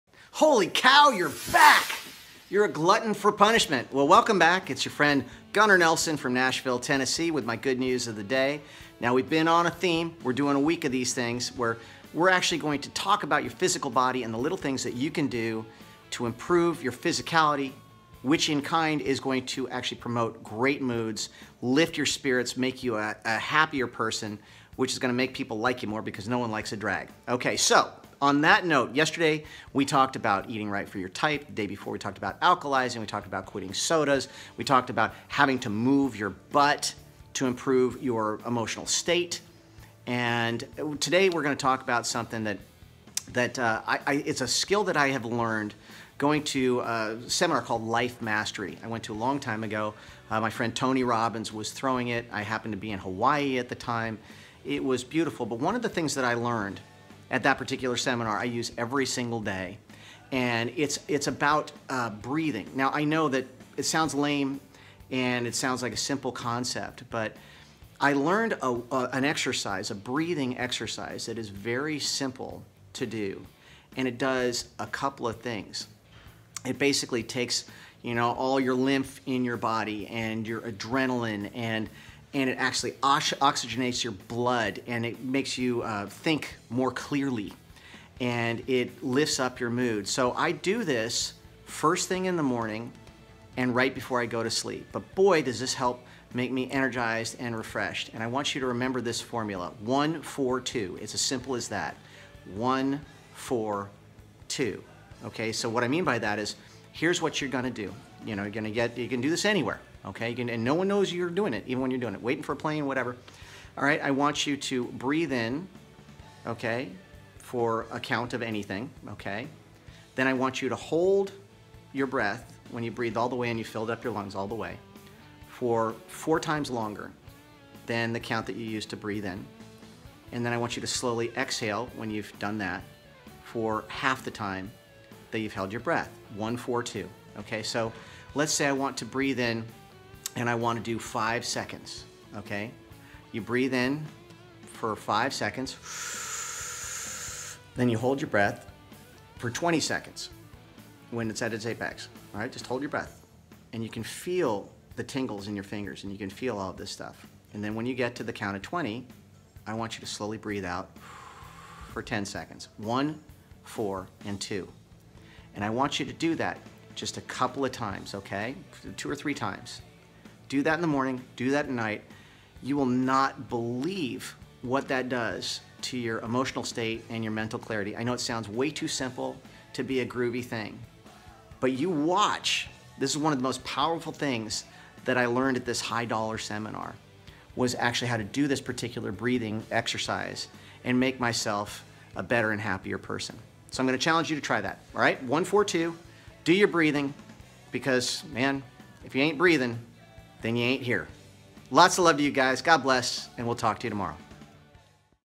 Good news from Gunnar episode 172
This technique is some thing I learned from my friend Tony Robbins at a seminar I was lucky enough to attend in Hawaii. It’s all about breathing and oxygenating the blood and the only thing you need to remember is one, four, too. Watch the video and I’ll teach you the trick.

To have Gunnar create your own personalized CameoVideo navigate to his